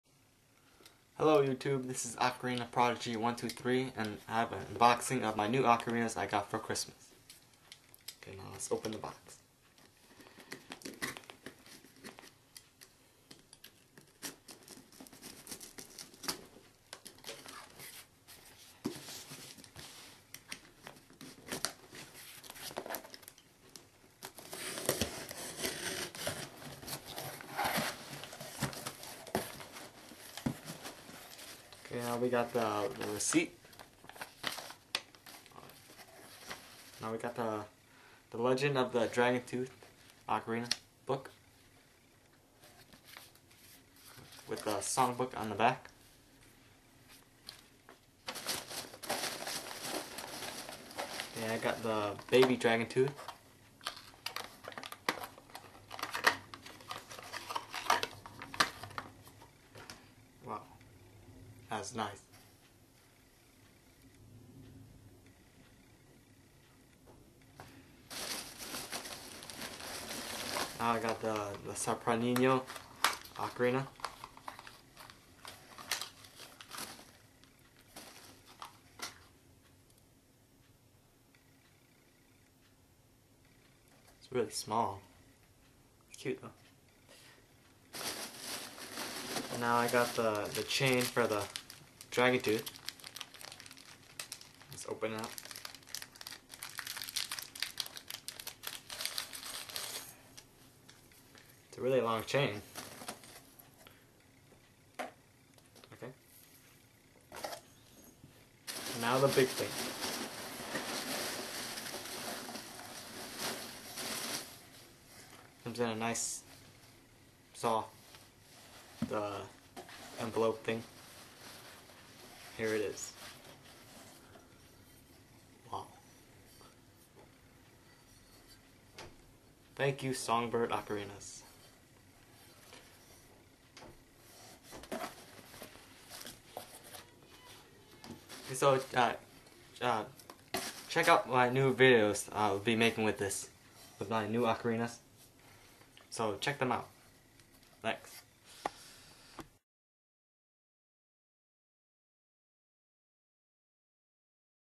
Songbird Ocarinas Unboxing
Unboxing video of ocarinas I got for Christmas.

Ocarinas:
Dragon Tooth Alto C in Strawfire
Baby Dragon Tooth Ocarina in Strawfire
Sopranino Ocarina in F